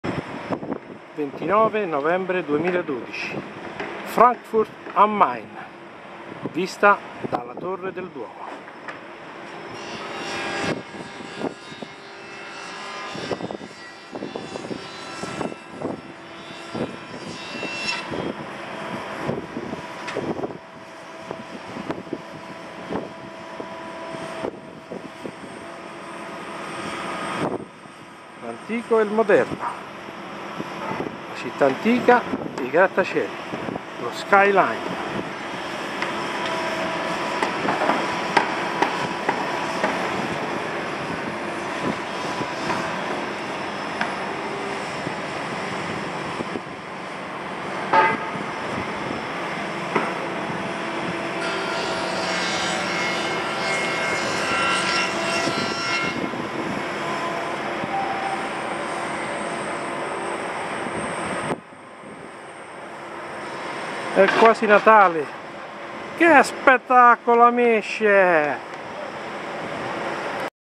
Frankfurt am Main
Francoforte vista dalla torre del Duomo